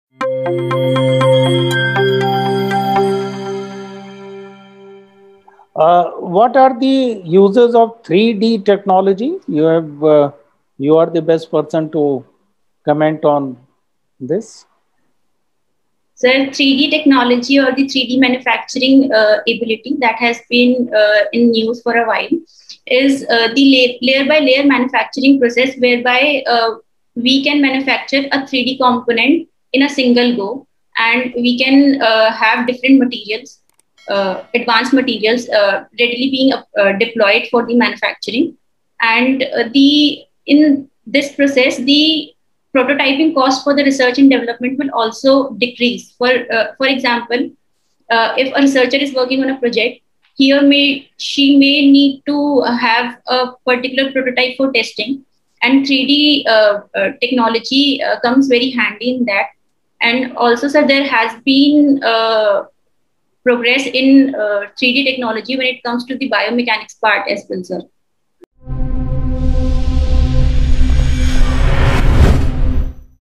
What are the uses of 3D Technologies? | Vaishali Jain | UPSC-IAS Mock Interview
In this video, Vaishali Jain (AIR 2z - IAS/UPSC 2020 Topper) will Share her views on the Uses of 3D Technologies.

Chahal Academy: A Socially Responsible Institute.
CHAHAL’s CSR INITIATIVE :-
9. Facts of the day
Follow us for regular updates :-